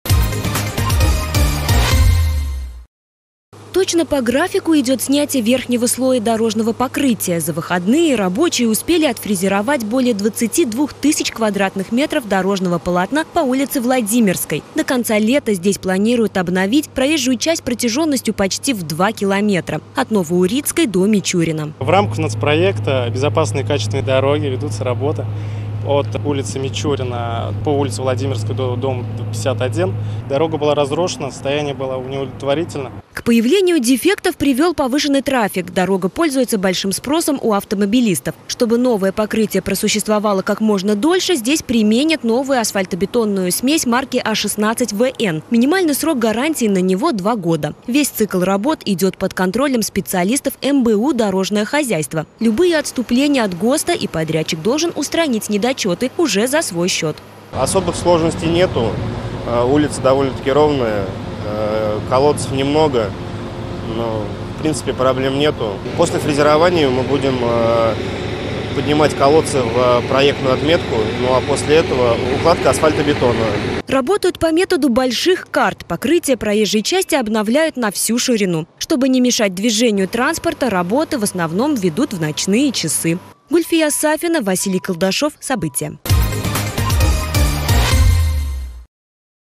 В столице региона продолжается масштабный ремонт дорог
В столице региона продолжается масштабный ремонт дорог. Сейчас работы идут на улице Владимирской. Модернизацию дорожного полотна ведут в рамках национального проекта "Безопасные качественные дороги". Участок от Ново-Урицкой до Мичурина обновят методом "больших карт". За выходные специалисты успели отфрезеровать почти 2 километра полотна. За ходом работ вместе с экспертной группой наблюдала Гульфия Сафина.